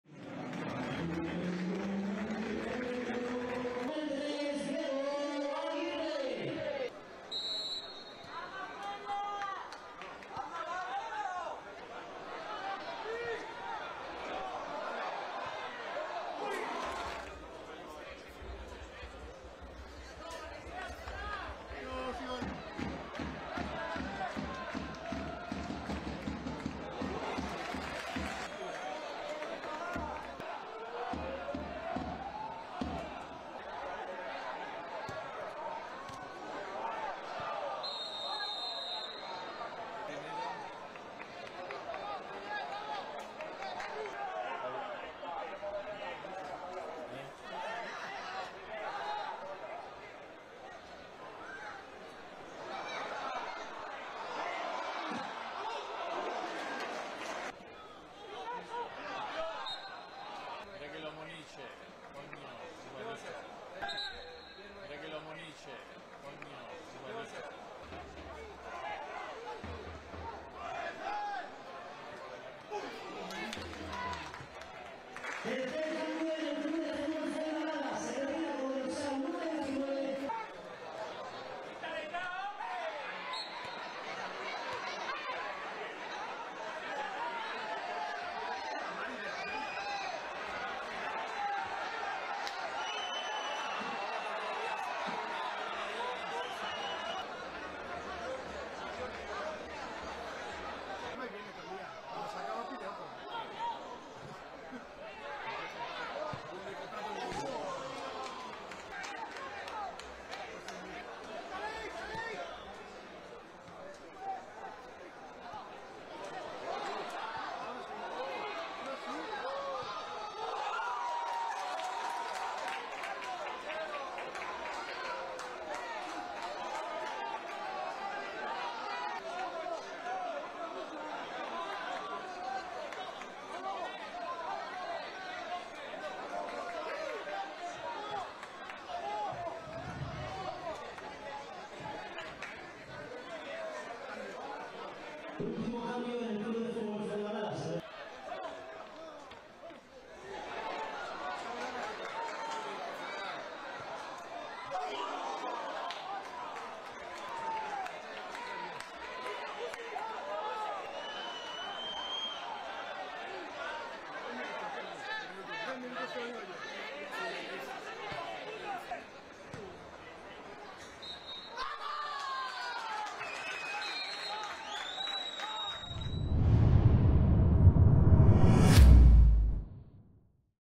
RESUMEN PrimeraFederación | CF Fuenlabrada 0-1 AD Mérida I Grupo 1 I Jornada 15
⚽ 0-1 | Lolo Plá (85')

📍 Fernando Torres